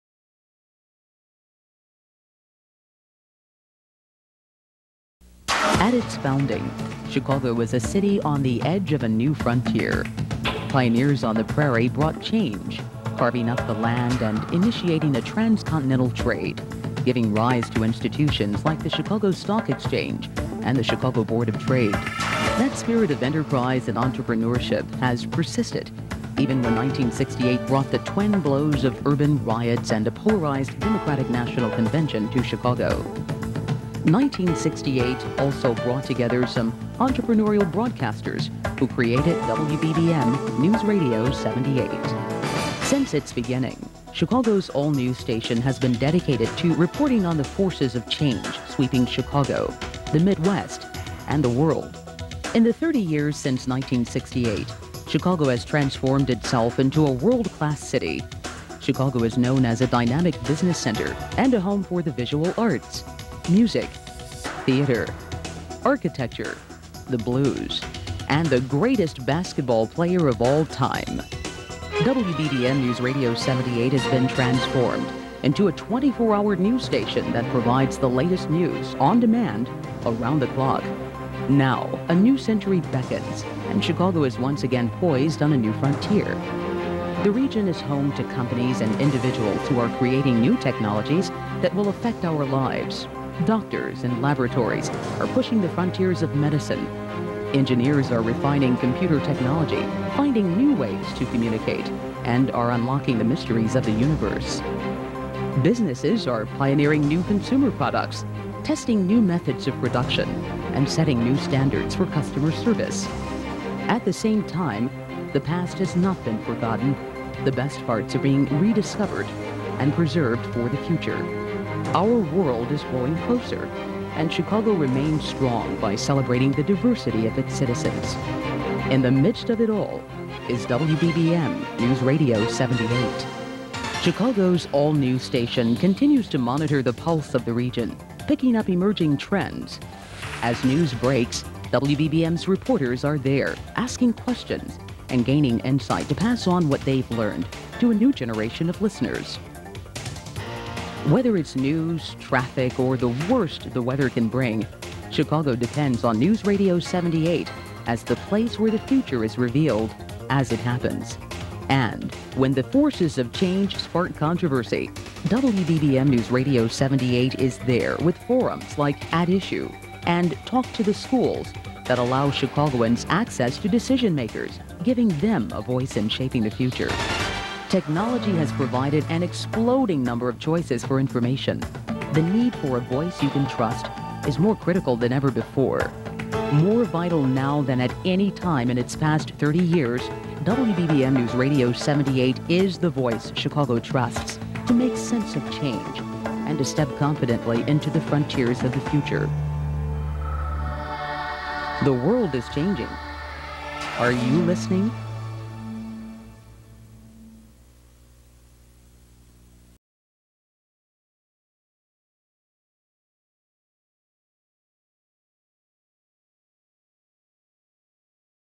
WBBM Newsradio Promo Video (Late 90s)
This is from the late 90s I think. Felica Middlebrooks narrates this promotional video that coincided with Newsradio's campaign - "The World is Changing, Are you Listening"